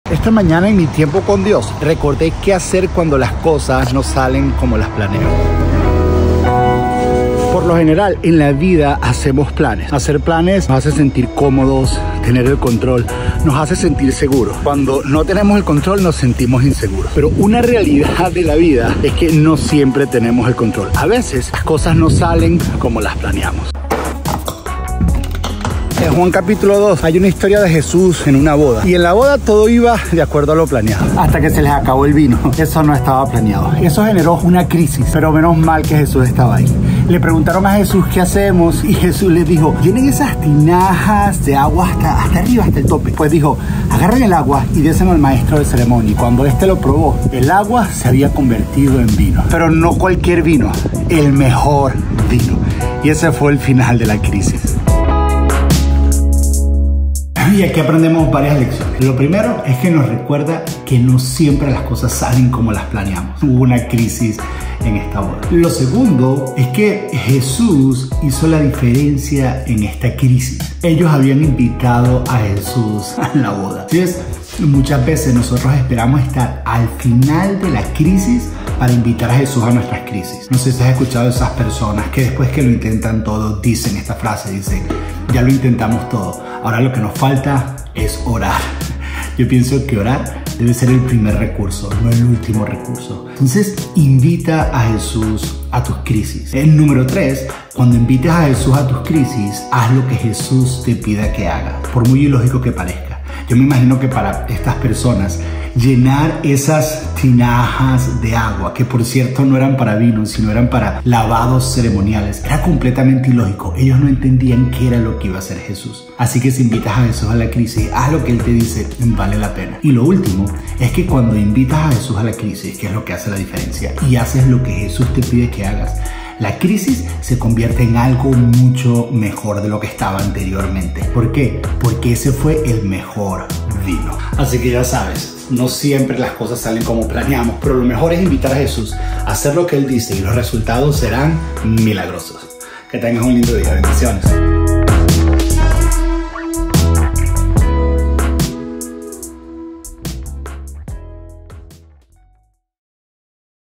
QUE HACER cuando los LOS PLANES se interrumpen
Descubre lecciones valiosas de la historia de Jesús en las bodas de Caná. En este video, exploramos cómo las crisis pueden surgir inesperadamente en nuestras vidas y cómo invitar a Jesús puede marcar la diferencia. Aprende cómo las adversidades pueden convertirse en oportunidades para la intervención divina.